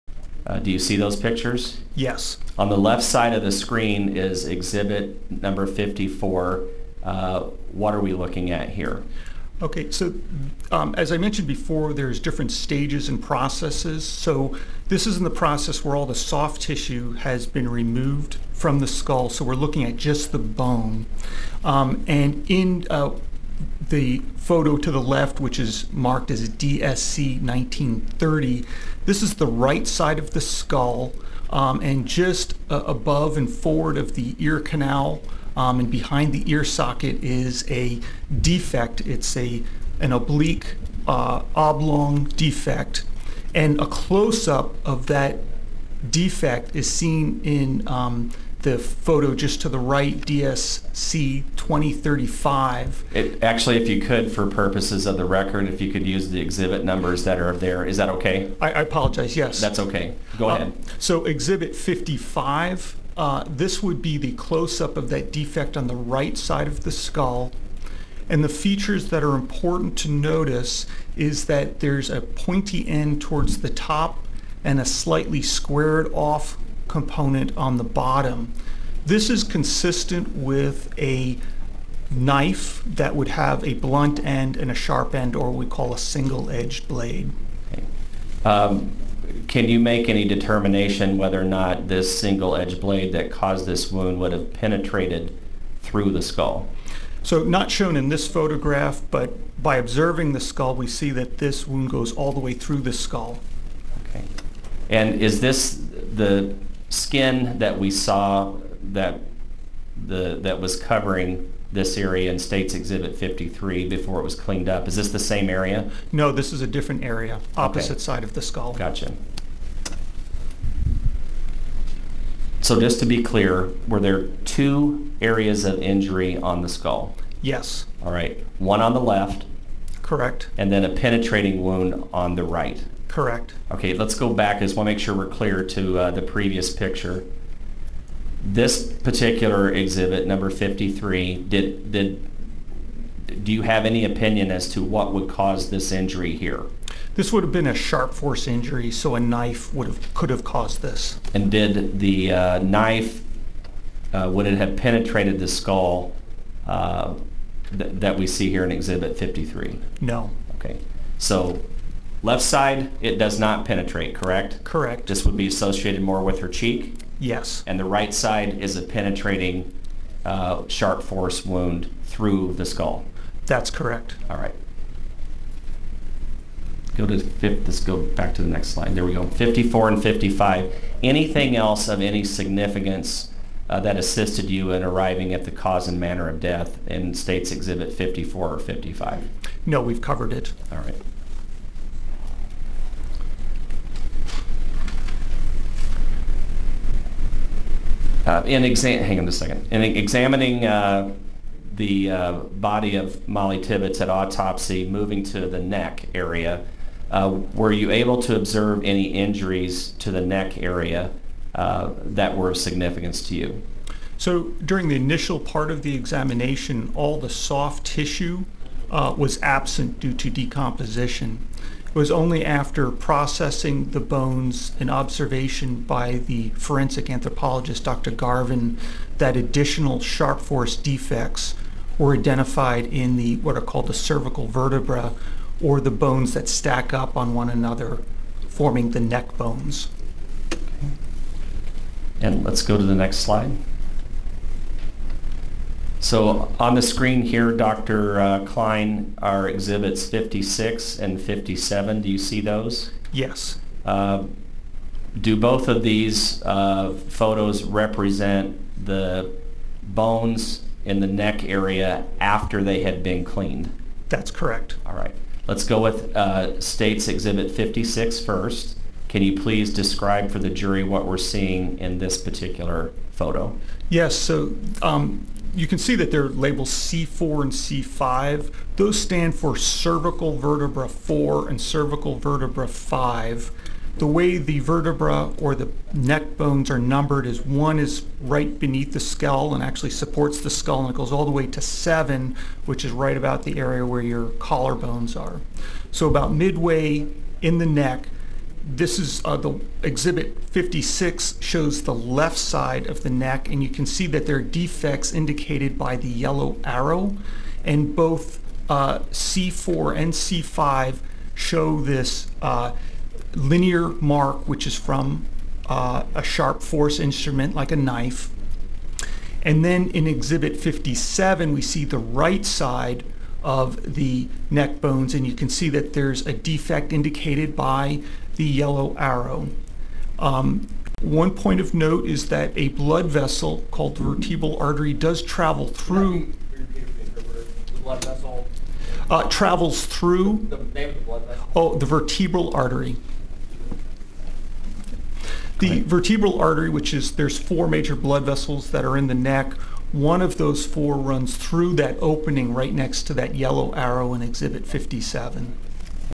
Medical Examiner Describe the Injuries Suffered by Mollie Tibbetts | COURT TV
THE KILLING OF MOLLIE TIBBETTS: The state has rested their case in the murder trial of CristhianBahenaRivera.

Before wrapping their case, the medical examiner who conducted the autopsy on MollieTibbetts took the stand to describe her injuries.